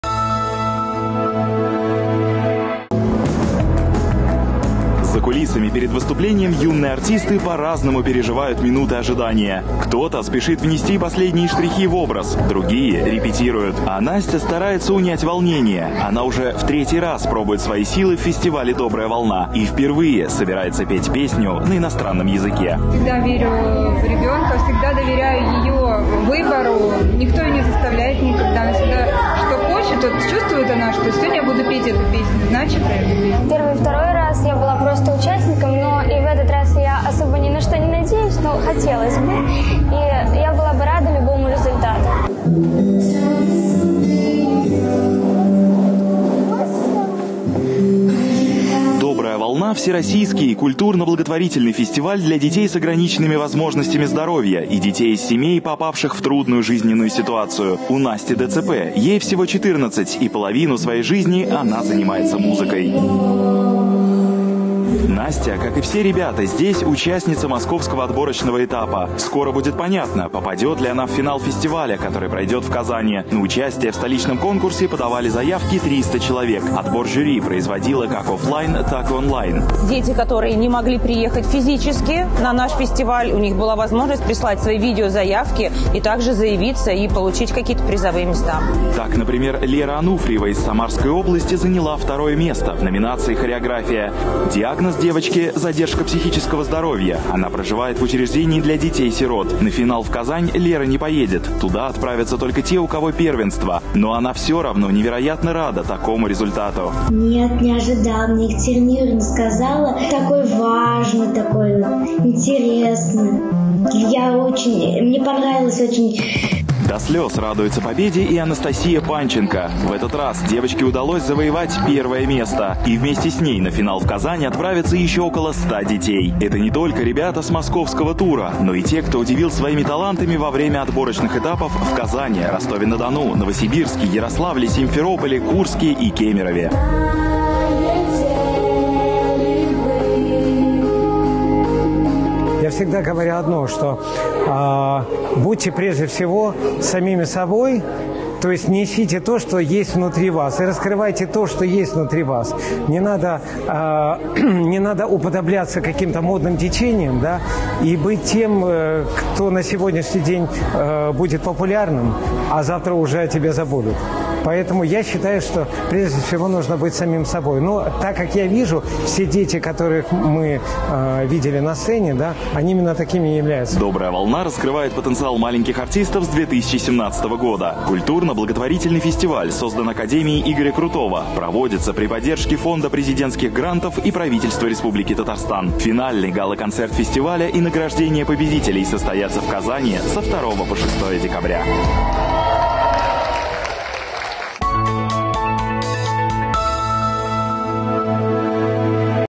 Региональный этап благотворительного фестиваля «Добрая волна» в Москве
17 октября 2021г.
В Москве прошел региональный отборочный тур Всероссийского культурно-благотворительного фестиваля детского творчества «Добрая волна» при поддержке Фонда президентских грантов. В фестивале принимали участие талантливые дети с ограниченными возможностями и дети, находящиеся в трудной жизненной ситуации.

Отборочный тур и праздничный концерт проходили во дворце культуры МГТУ имени Н. Э. Баумана. Всего на конкурс было подано более 400 заявок: участников и творческих коллективов из Москвы и Московской области. Во время отборочного тура 16 октября профессиональные члены жюри оценивали творческие номера детей и подростков от 6 до 16 лет. Юные артисты показывали свои таланты в четырёх направлениях фестиваля: музыка (вокал), игра на инструменте, хореография и сценическое искусство (оригинальный жанр, чтение, жестовое пение). Более 50-ти заявок были поданы онлайн и члены жюри смотрели эти номера на видеоэкране.

Во второй день фестиваля — 17 октября — прошел праздничный концерт и награждение победителей. Одиннадцать юных артистов (в их числе дуэт и танцевальный коллектив) поедут в начале декабря в Казань, чтобы участвовать в финальном этапе конкурса и побороться за призовые места.

Анастасия Панченко, победительница «Доброй волны» в номинации «МУЗЫКА», возрастная категория 13-16 лет: — Эта награда для меня большой подарок. Я уже третий год участвую в «Доброй волне» и вот, наконец, победила. Я так рада, что поеду в Казань на финал конкурса. Это непередаваемые эмоции! Спасибо моим педагогам и родителям за поддержку и веру в меня.

Юлия Панченко, мама Анастасии Панченко: — Мы шли к этой победе очень долго — целых три года. Эмоции переполняют: сердце от волнения и радости колотится. Огромное спасибо организаторам за такой замечательный проект, как фестиваль «Добрая волна». А деткам-участникам желаю никогда не останавливаться на достигнутом, двигаться уверенно вперед и обязательно верить в себя.

В рамках фестиваля с 19 по 25 октября будут проведены профессиональные мастер-классы с лучшими педагогами. В связи с эпидемической обстановкой они пройдут в онлайн-формате на YouTube-канале «Доброй Волны». Поучаствовать в мастер-классах смогут дети из любых городов России.

Марк Сандлер, член жюри, джазовый пианист, аранжировщик, педагог, профессор Пекинской академии искусств, вдохновитель и организатор движения а капелла в России (с 1994 года): — Я очень рад, что такой проект, как «Добрая Волна», живет и развивается. Он мотивирует наших детей двигаться с невероятной скоростью, несмотря на жизненные трудности. Желаю им не следовать модным влечениям, а быть прежде всего самими собой: нести и раскрывать то, что у них внутри.

Антон Азаров, член жюри, автор и исполнитель собственных песен, руководитель благотворительных проектов фонда «Северная корона»: — «Добрая волна» — большой всероссийский праздник. Он потрясает и не может не трогать человеческие сердца. Быть членом жюри на этом проекте не просто — это огромная ответственность. Ты видишь талантливых ребят, у которых всё еще впереди, но у каждого есть ограничение по здоровью или какая-то другая трудная жизненная ситуация. Это заставляет заново посмотреть на их творчество, открыть совершенно новые краски. Много детей, которые приходят на конкурс не в первый раз. Большая радость наблюдать, как они растут.